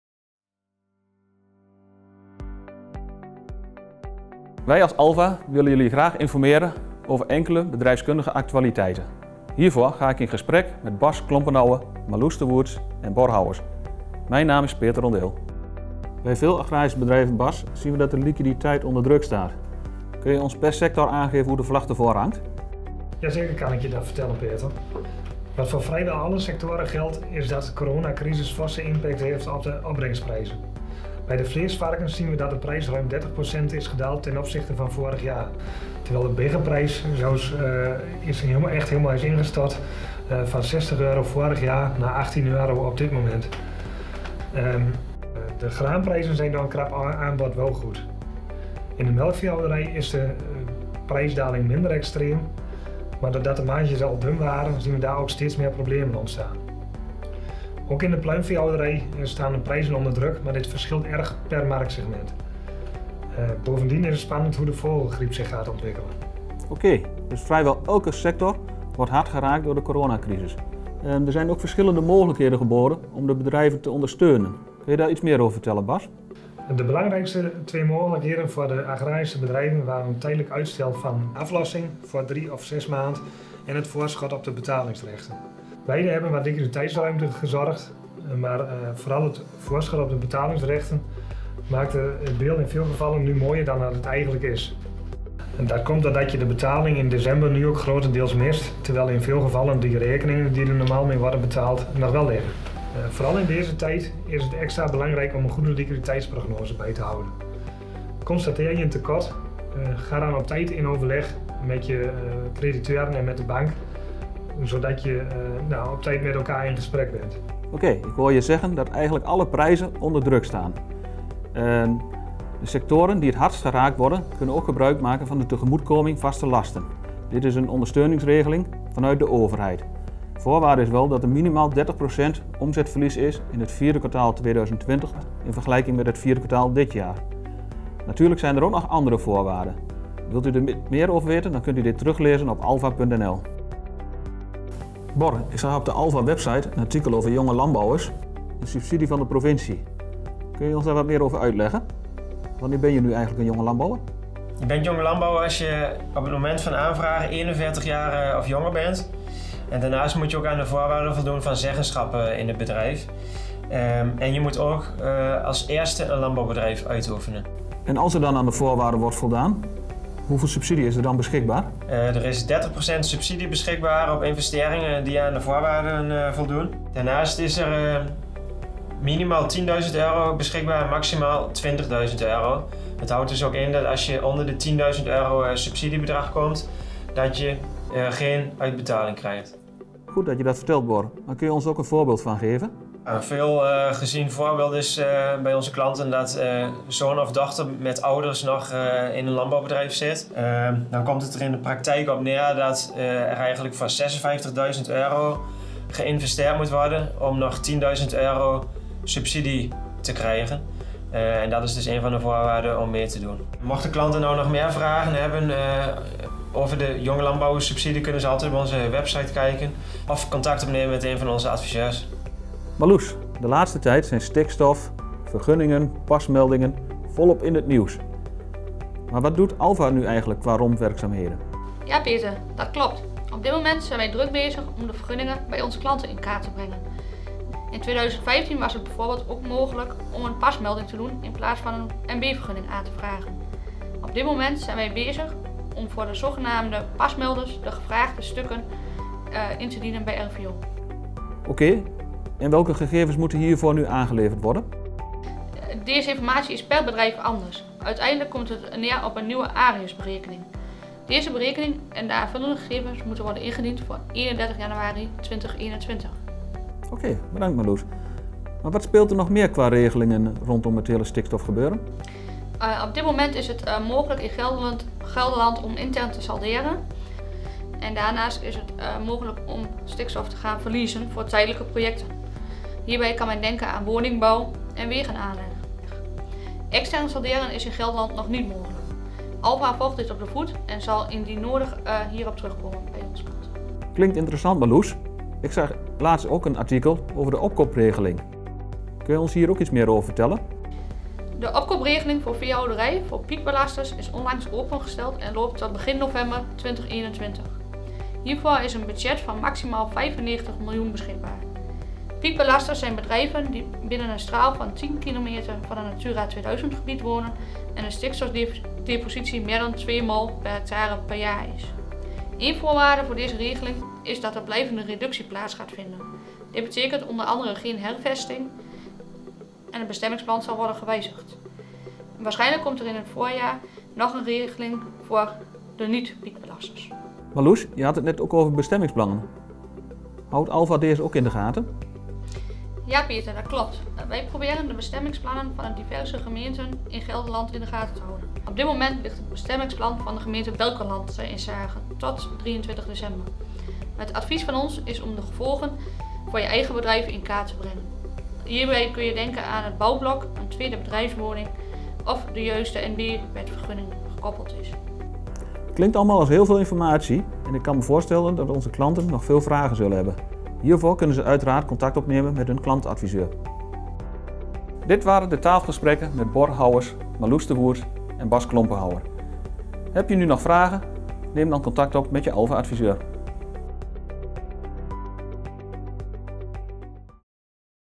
Alfa Agro Tafelgesprek - Achterhoek
Wij willen je graag met deze video informeren over bedrijfskundige actualiteiten. Peter Rondeel is hiervoor in gesprek gegaan met adviseurs Bas Klompenhouwer, Bor Houwers en Marloes ter Woerds. Zij bespreken onder andere financiële trends, ondersteuningsmaatregelen en de stikstof regelgeving. Kijk je mee?